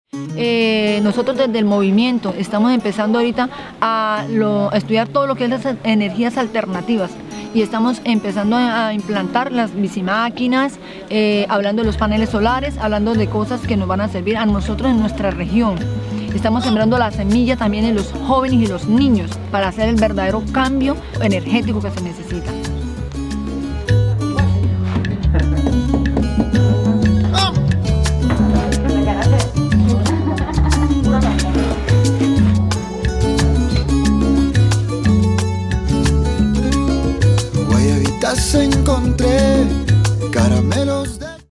3 Palabras comunitarias para la Cumbre del Clima COP 26
Mientras  gobiernos y empresas proponen falsas soluciones para afrontar la crisis climática, las comunidades con alternativas y saberes populares ponemos en el centro la vida y no el dinero.